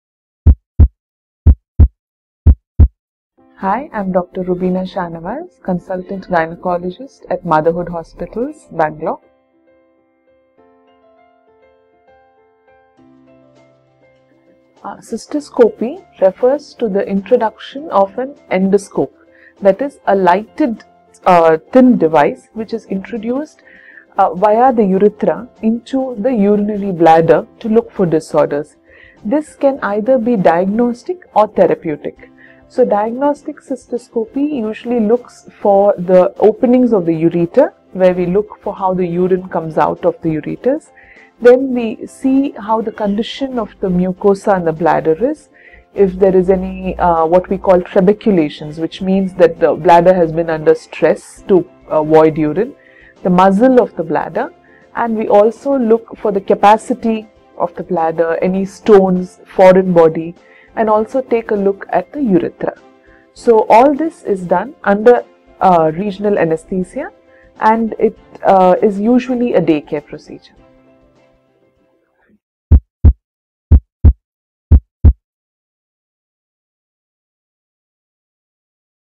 What is cystoscopy? - Dr. Rubina Shanawaz
Cystoscopy refers to the introduction of an endoscope i.e, a light, thin device which is introduced via the urethra into the urinary bladder to look for disorders. This can be either diagnostic or therapeutic. So diagnostic cystoscopy usually looks for the openings of writer where we we look how the urine comes out of the arts. If the bladder is under stress to avoid urinating and also take look at the urethra.